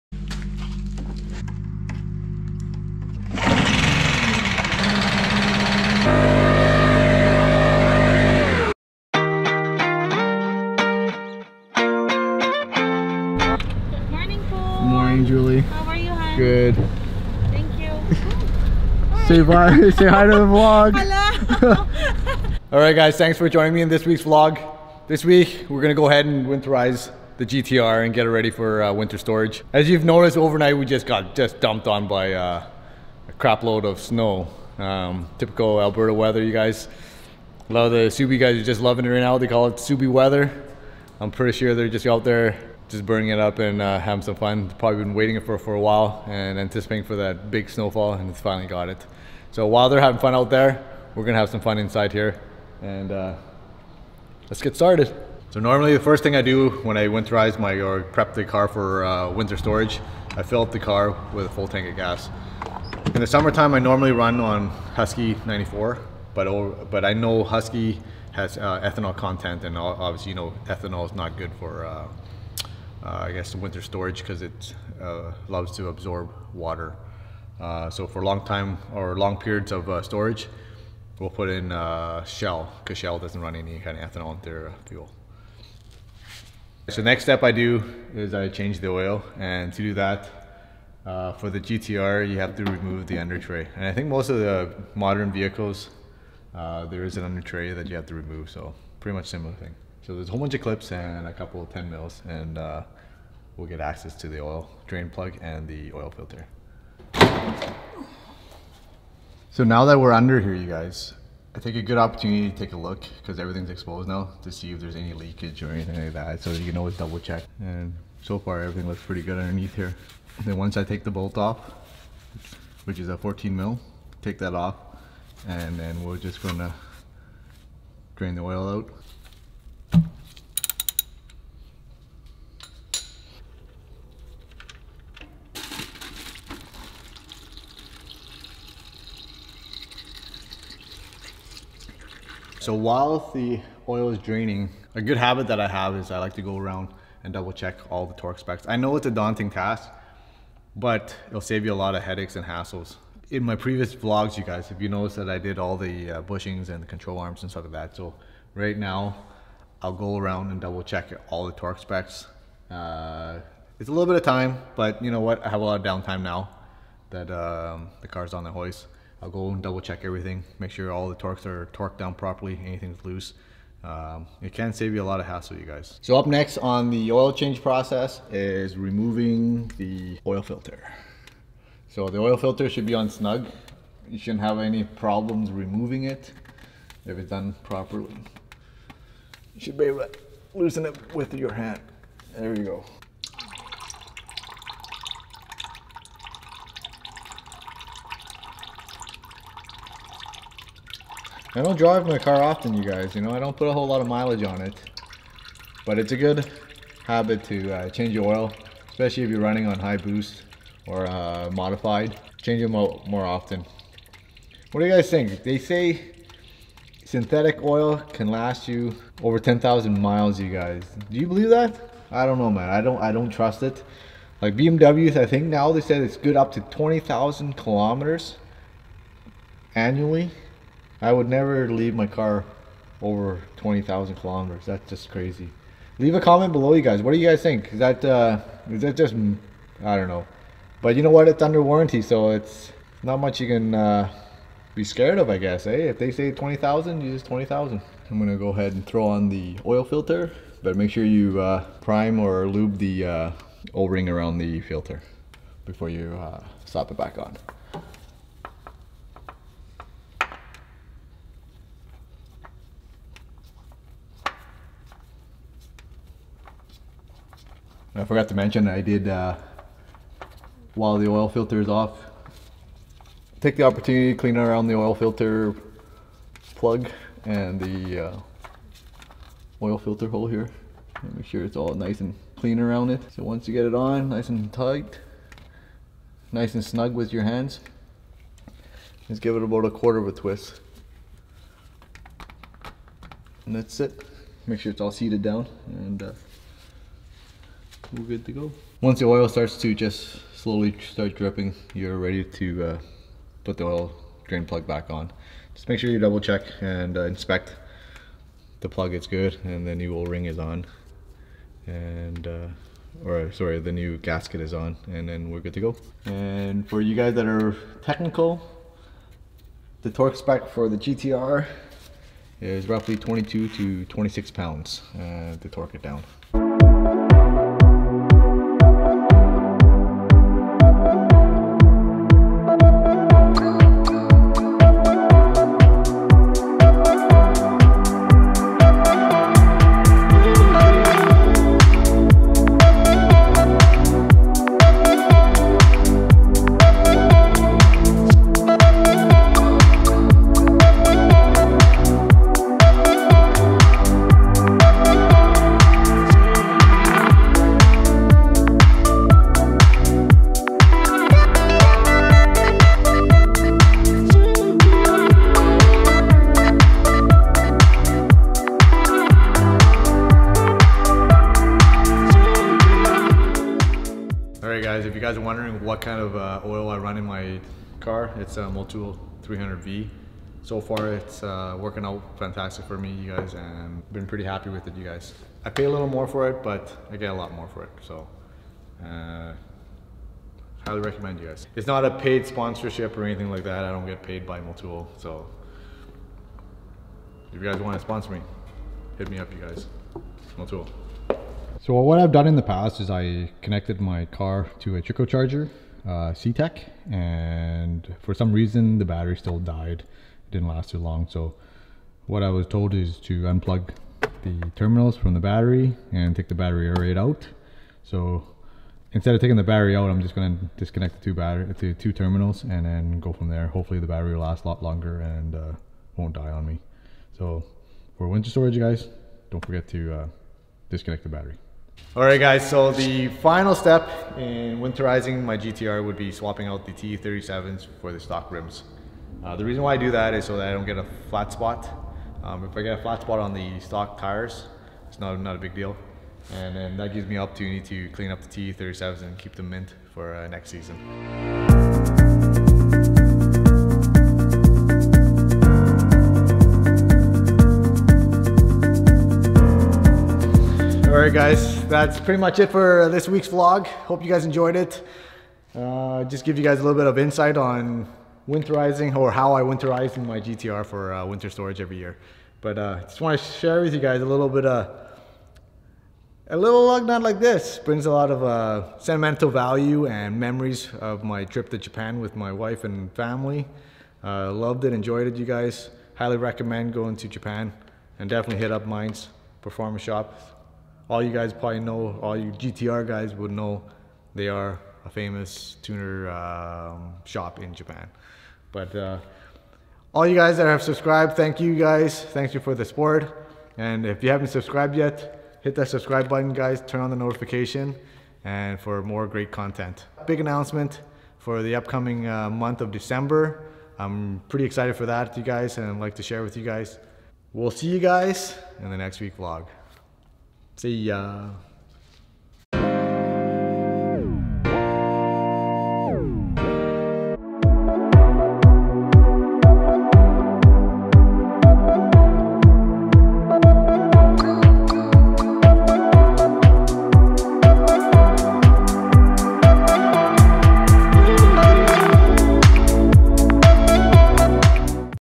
How I prep the GTR for winter.
This is how I get the GTR ready for the winter. I do this every fall to ensure there are no hiccups come spring time.